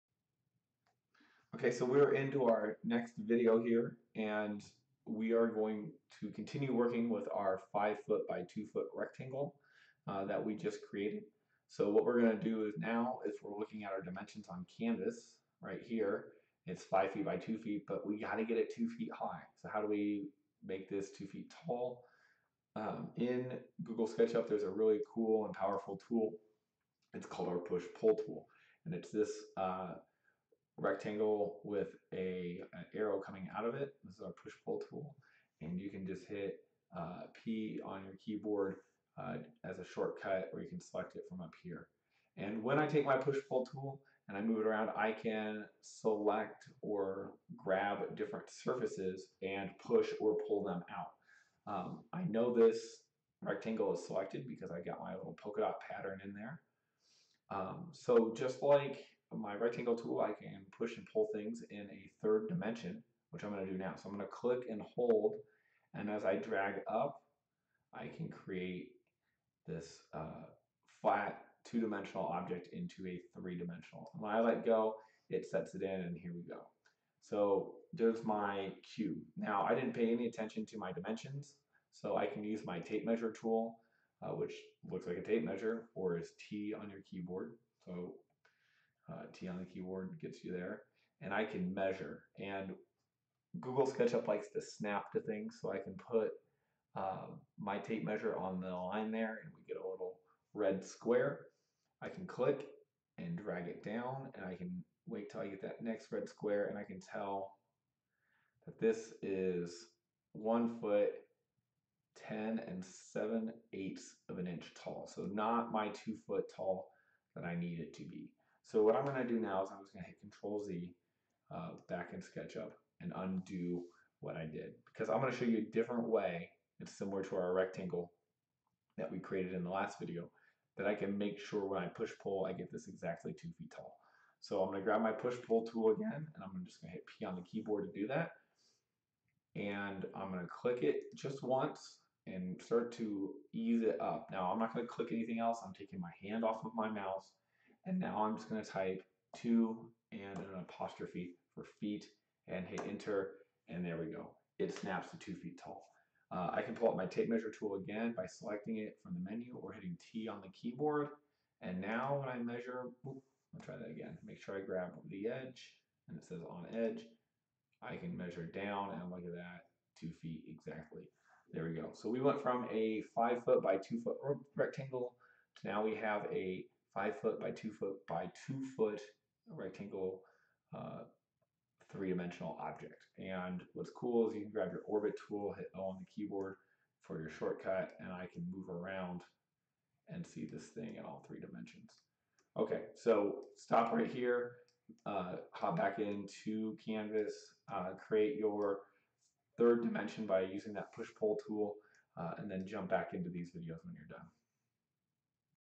Offset Ledge Step #2
This video shows basic use of the push/pull tool in google sketch up.